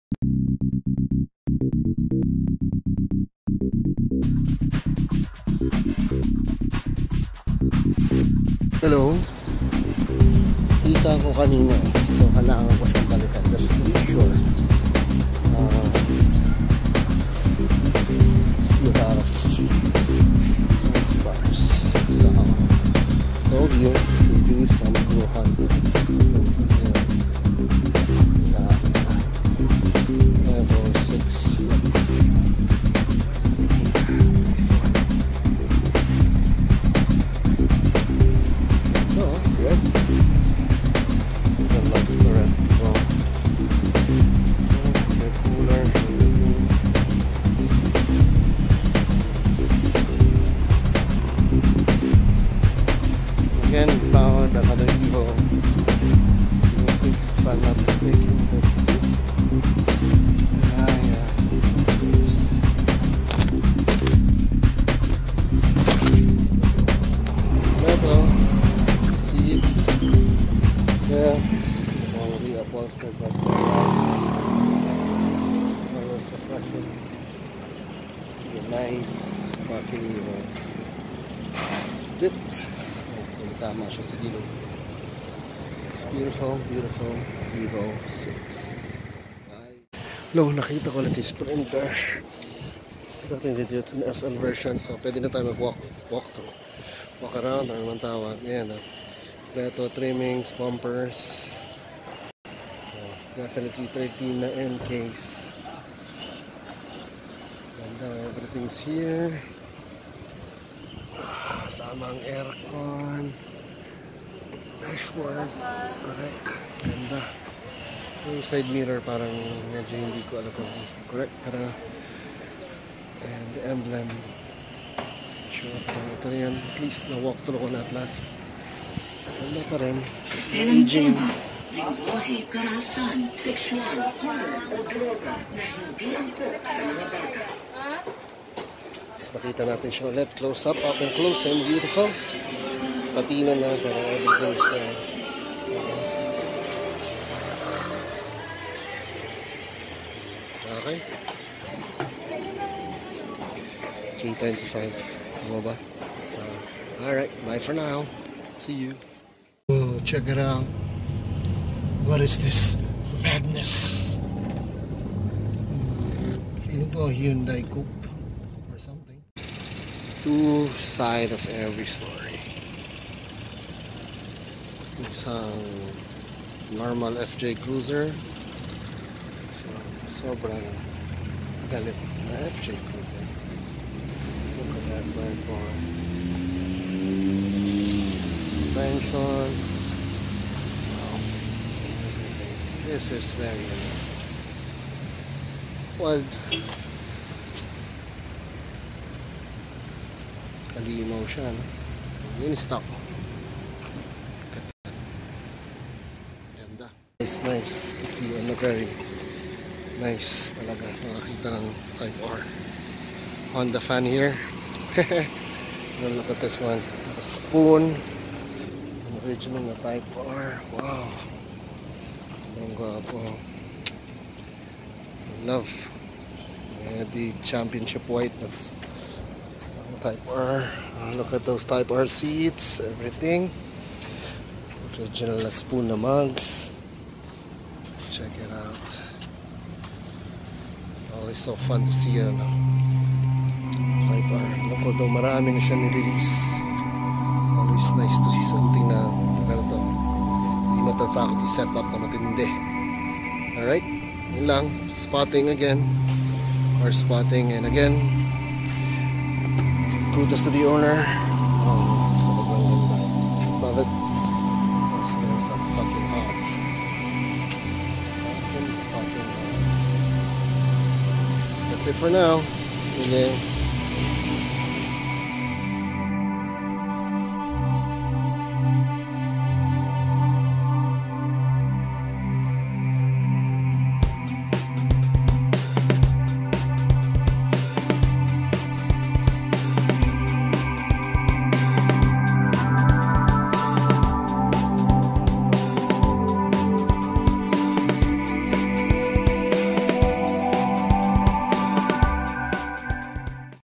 mitsubishi lancer Evo 6 plus sprinter fj cruisers and honda type r civic/carspotting tayo! ep : 8
Carspotting tayo ulit sir!! Spotted yun sprinter na dati kong nakita.nakatagong evolution 6 mga  fj cruiser at spoon na type r!